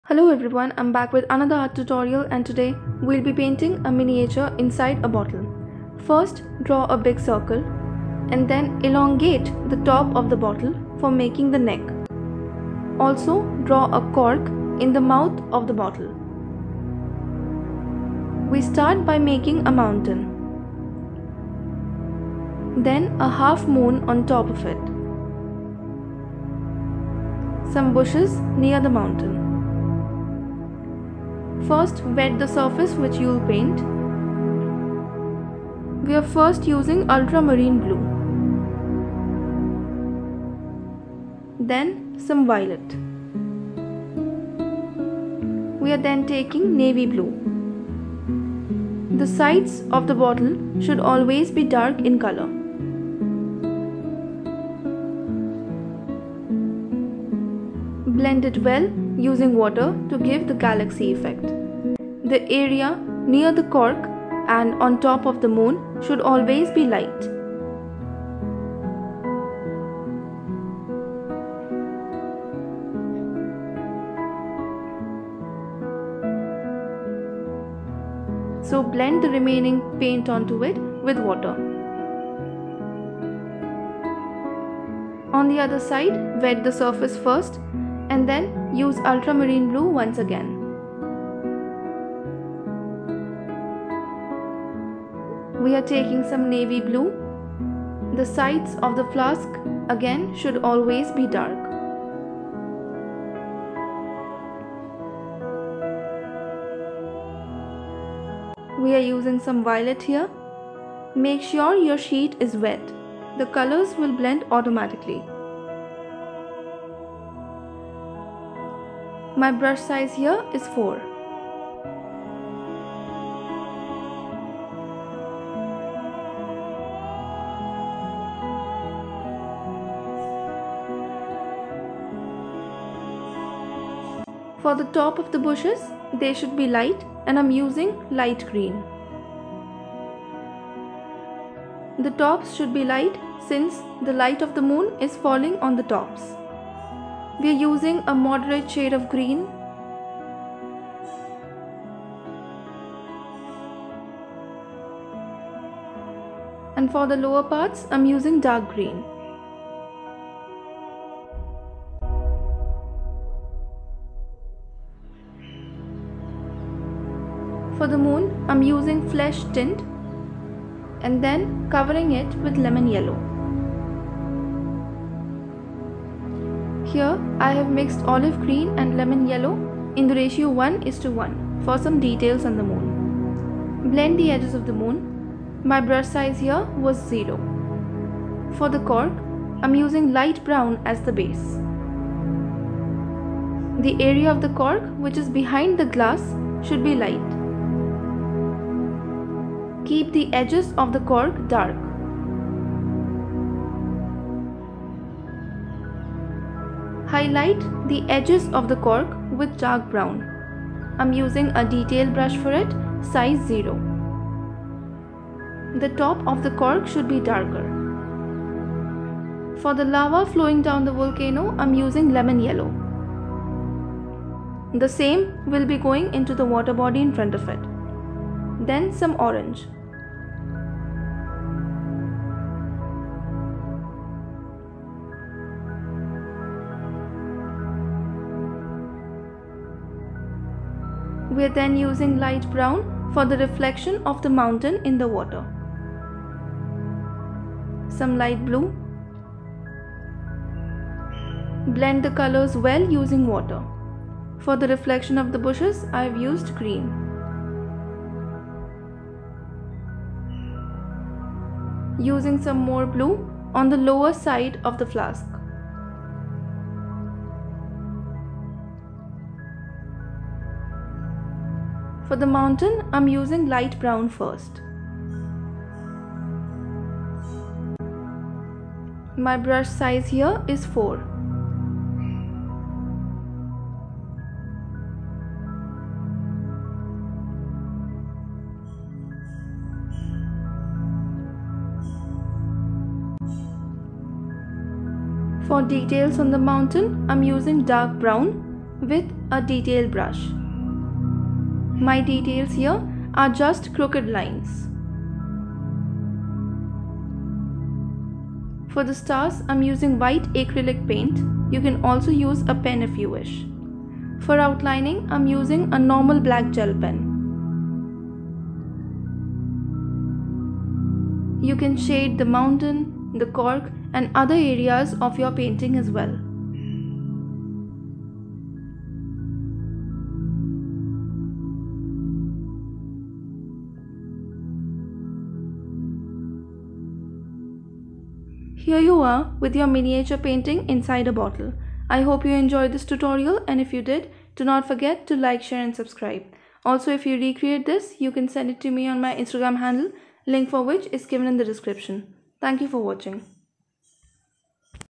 PAINTING A MINIATURE VOLCANO | Easy Step By Step Process With Watercolors
Hello everyone , this is a very easy tutorial with watercolors to  create a beautiful miniature volcano inside a bottle with a galaxy sky .
With watercolors, what you always need to keep in mind is the water quantity you use . Watch the video till the end for each and every detail of completing this painting .
This painting took me around 40mins to complete , so get your paint brushes out and let's go!!

I used a normal black gel pen for detailing .

I bought my brushes from a local art shop .

link to sketchbook

( It's not necessary to buy this expensive set , you can start with any student grade set)